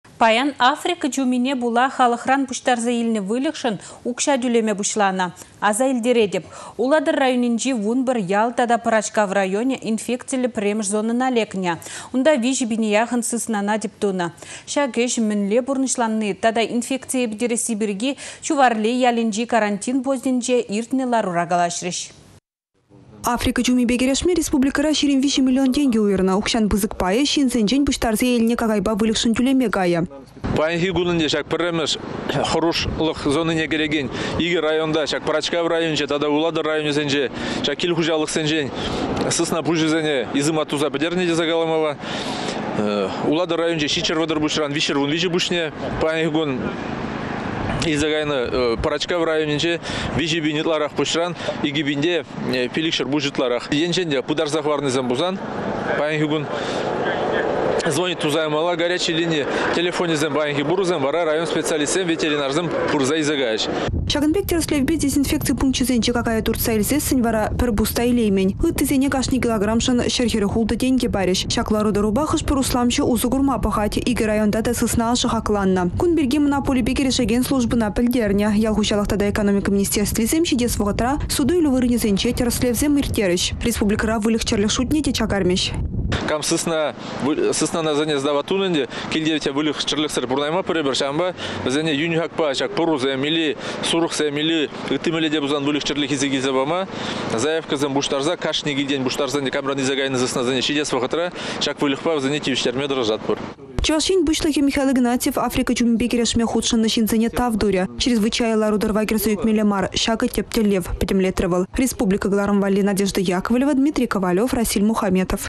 Африка чумине пĕтересси пирки
Паян Африка чумине пула халăхран пуçтарса илнĕ выльăхшăн укçа тÿлеме пуçланă. Аса илтеретĕп, Улатăр районĕнчи 11 ял тата Пăрачкав районĕ инфекциллĕ 1-мĕш зонăна лекнĕ. Унта 3 пине яхăн сыснана тĕп тунă. Çак ĕç мĕнле пурнăçланни тата инфекцие пĕтересси пирки Чуварлей ялĕнчи карантин постĕнче иртнĕ ларура калаçрĕç.